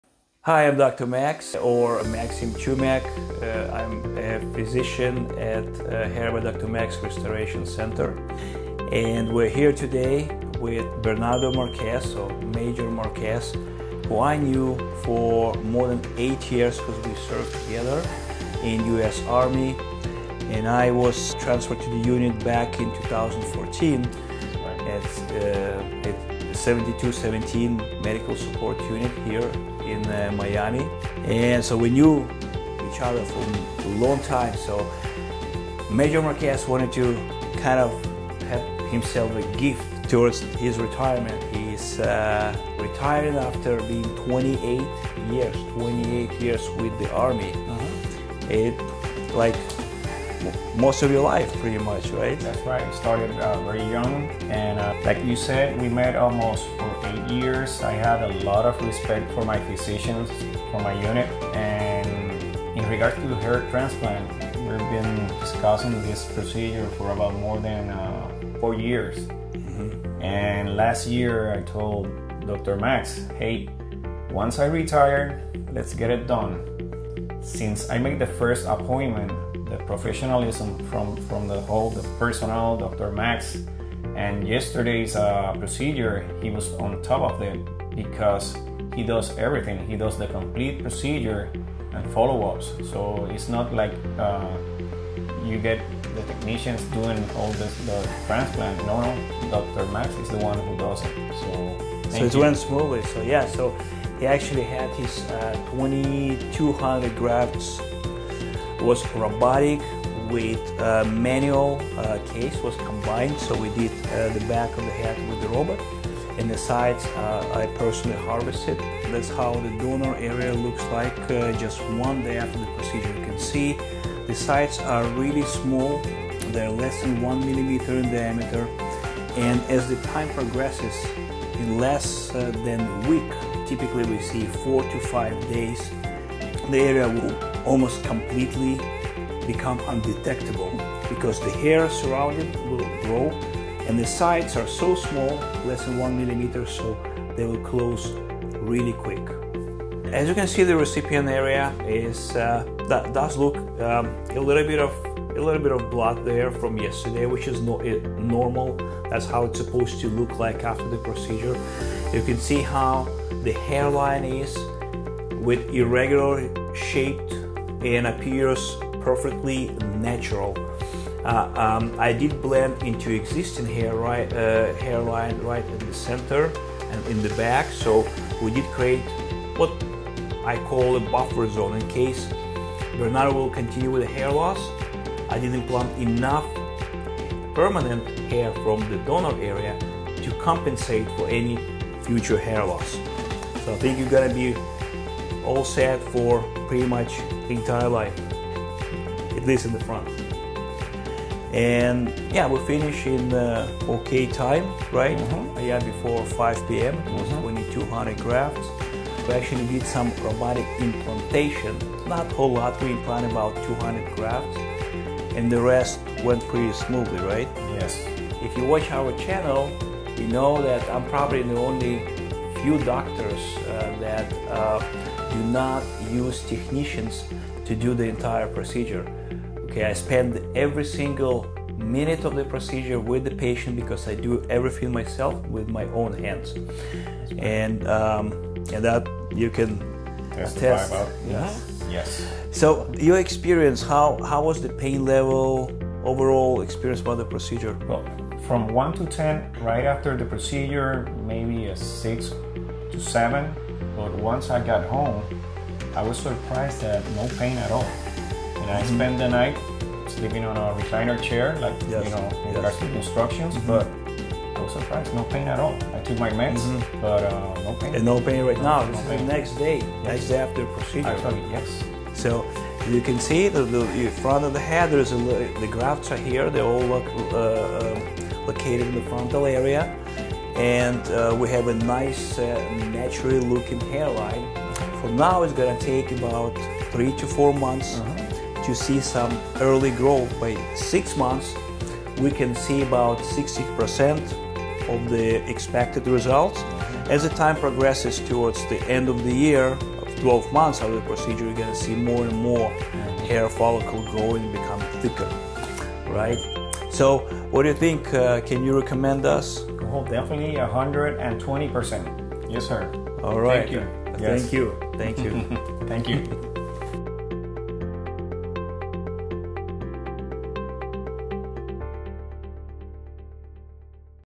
Combining Robotic and Manual FUE Hair Transplant Methods - Dr. Max
Bringbackhair.com

Dr. Max goes over a recent procedure in which both Robotic and Manual FUE hair transplant methods were used. The patient, an Army friend of Dr. Max, shares his hair transplant experience with us at Hair By Dr. Max, Restoration Center. We are proud to say that the procedure had done very well and we very much look forward to our patient's results in the coming months.